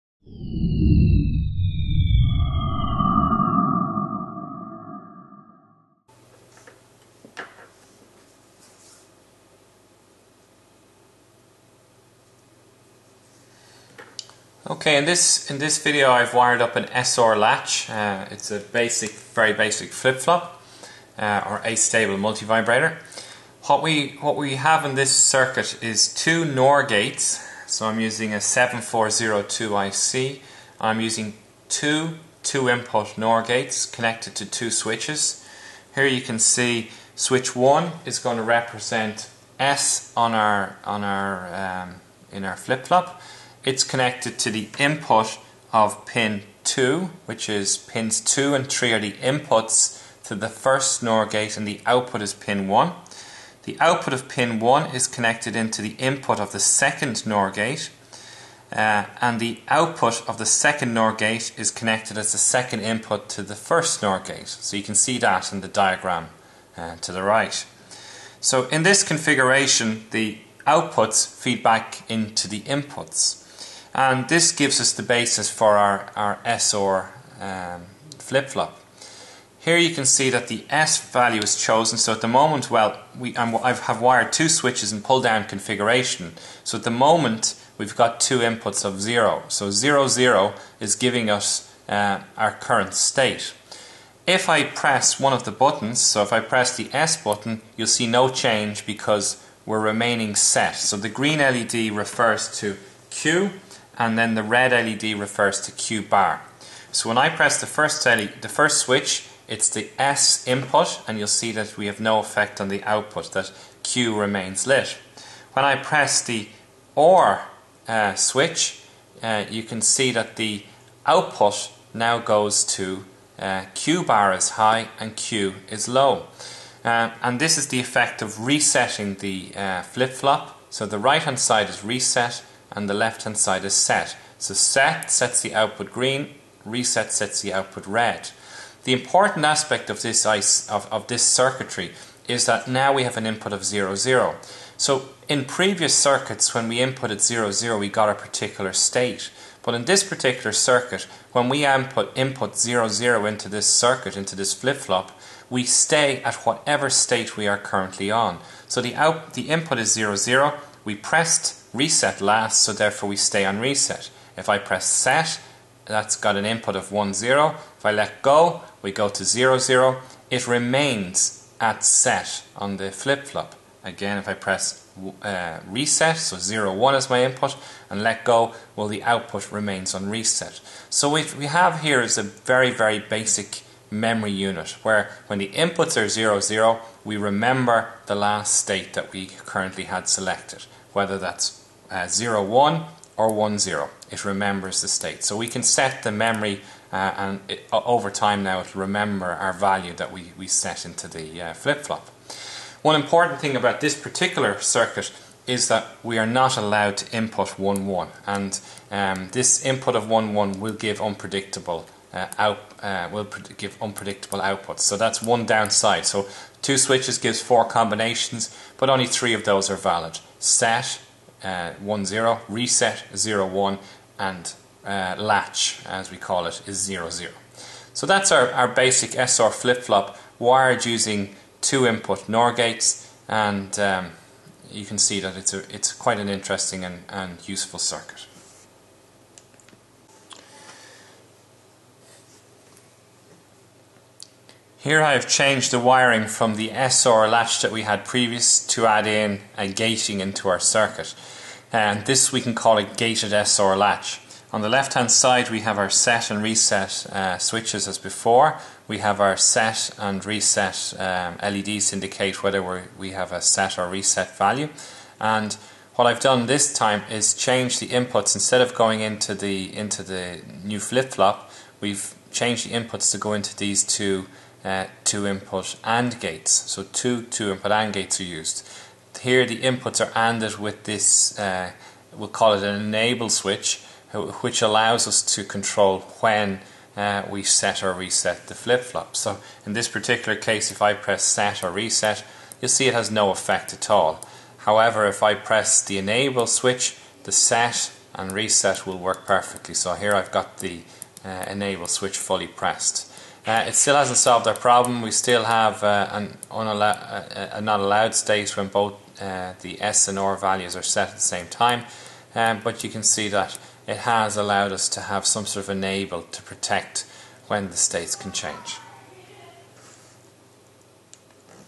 Experiments 3.1: Sequential Logic - S-R Latch and a Gated S-R Latch
This video demonstrates two circuits: First - an S-R Latch that is created using two NOR gates. It can be set or reset using the two inputs on the left hand side and shows how a basic flip-flop works. Second - a gated S-R latch that adds an enable input to the circuit, which must be pressed before the state can be changed. This is one of the first circuits in the EE223 Introduction to Digital and Analogue electronics module at Dublin City University.